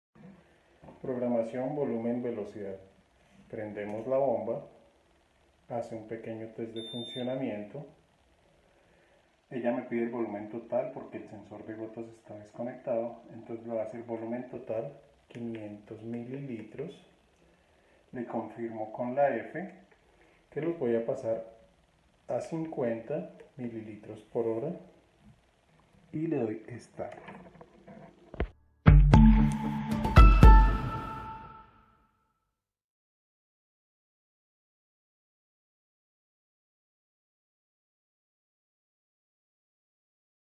6. Programación Volumen Velocidad - (Infusomat Compact)
Bomba de Infusión, Bomba Infusomat Compact ® de B. Braun Medical. En el video podemos apreciar la programación de volumen y velocidad de infusión (ml/h o cc/h) una infusión sencilla.